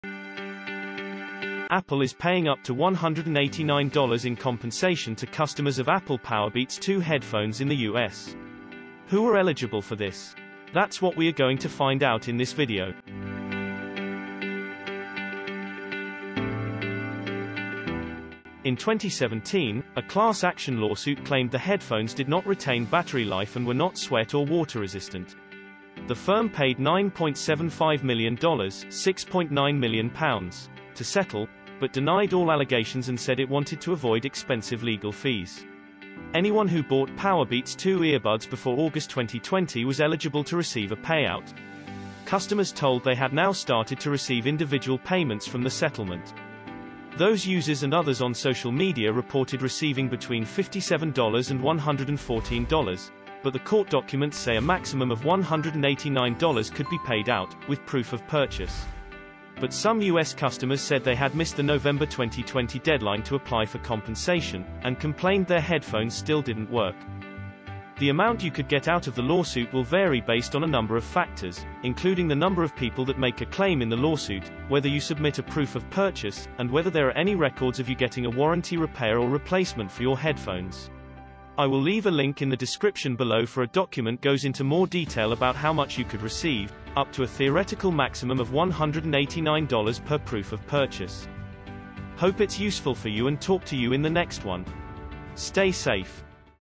Apple Powerbeats 2 headphone customers receive payout - $9.7 million
Apple is paying up to $189 (£133) in compensation to customers of Apple Powerbeats 2 headphones in the US.